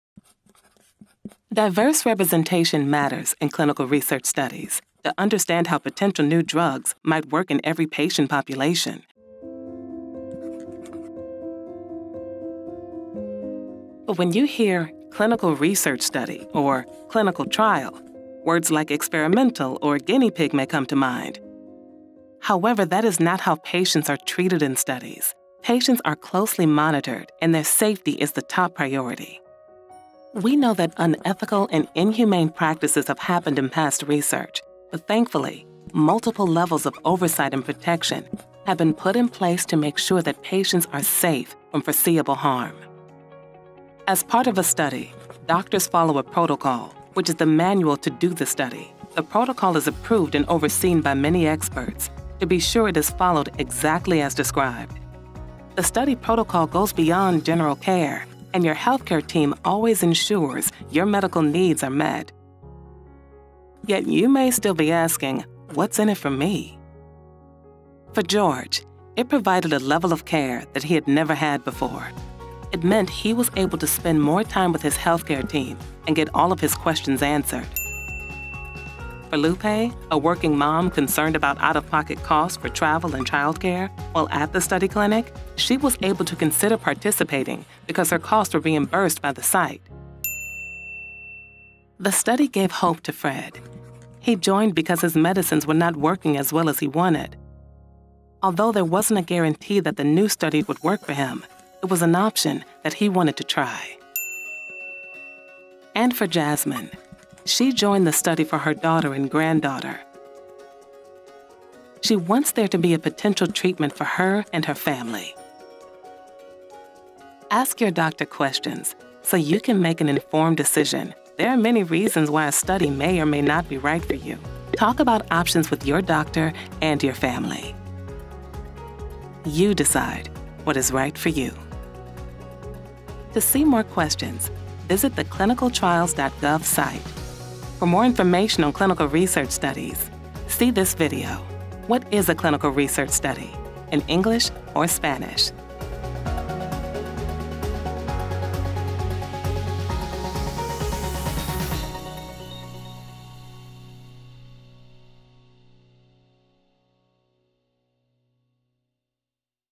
Is a Clinical Research Study Right for Me?
Clinical research studies, also known as clinical trials, help us develop medicines that are safe and effective. This video provides information to help patients make informed decisions about whether they should consider a clinical research study. Learn more about study safety, potential patient benefits, and the questions to ask your doctor about clinical research.

Want to hear more about what a clinical research study really is? Check out the video “What is a Clinical Research Study?"